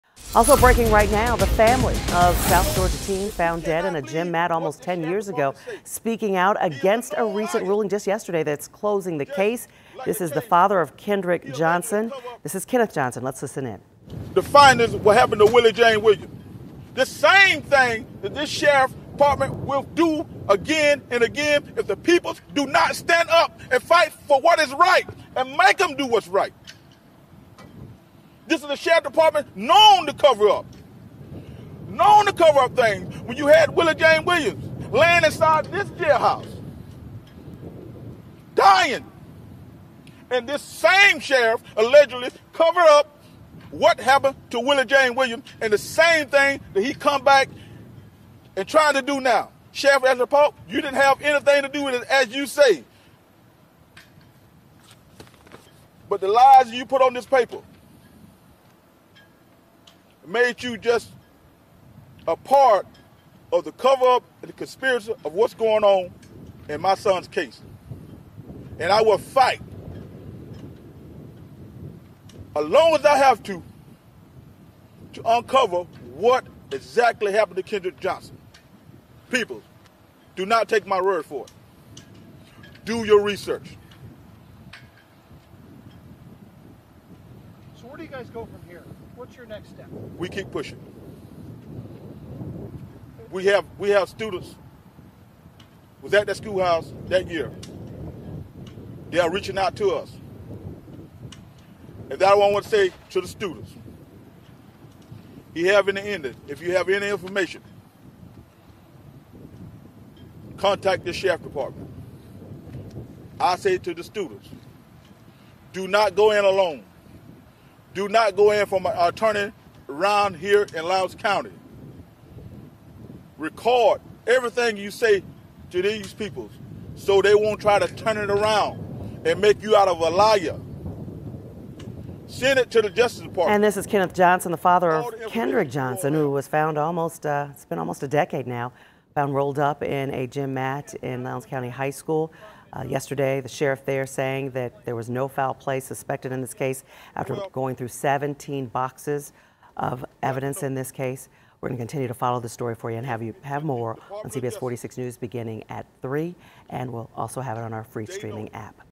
Kendrick Johnson's family speaks out on ruling in son's death investigation
Johnson's family argues that their son was murdered. STORY